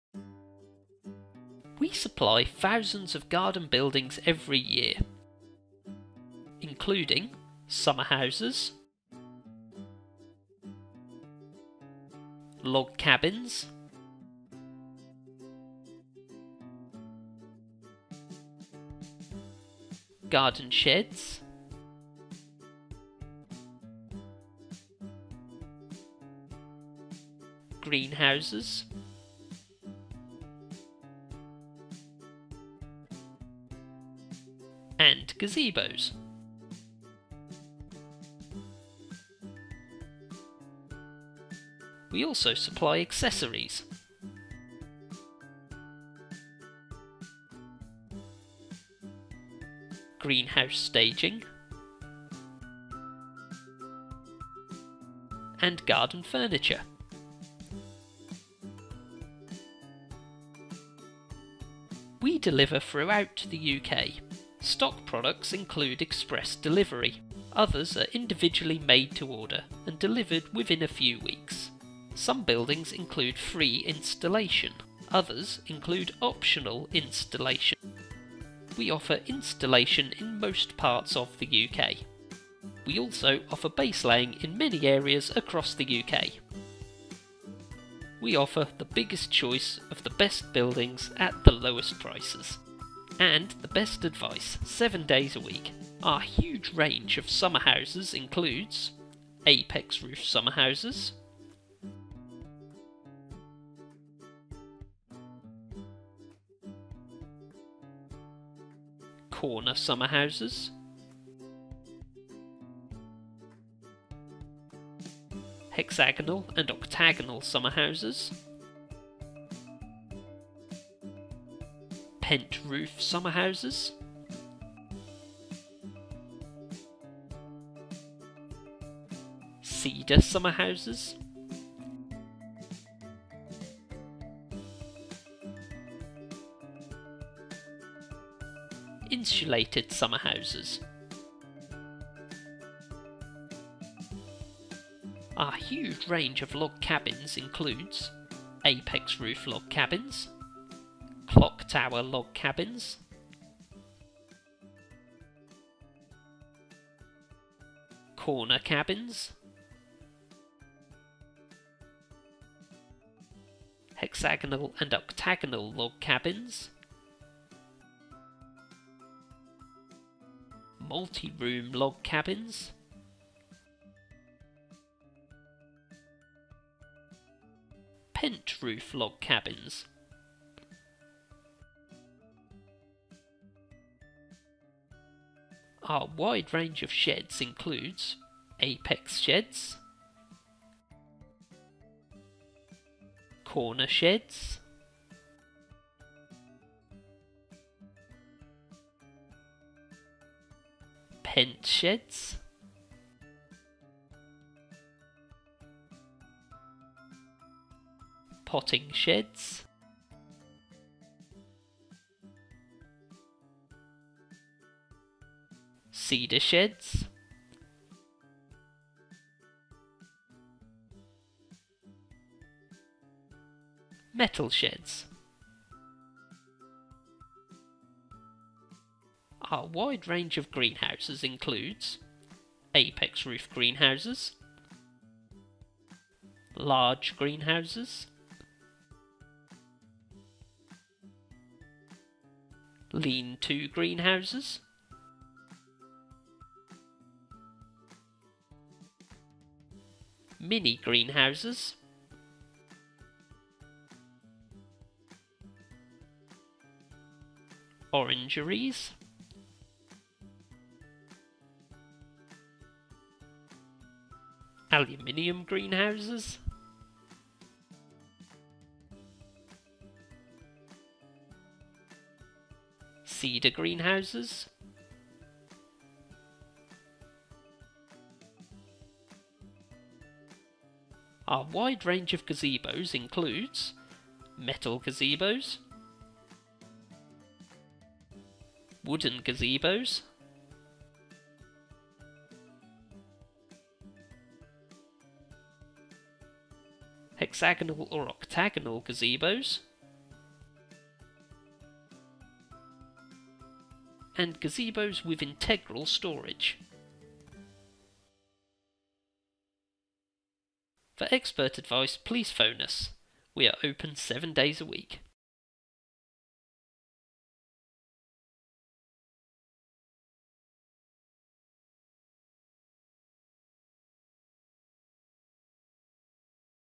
Summer Garden Buildings - An Introduction
Summer Garden Buildings is a leading UK supplier, established since 1981. We deliver garden buildings throughout the UK including sheds, summerhouses, log cabins, garages, greenhouses and gazebos.

to view our huge range of specialist garden buildings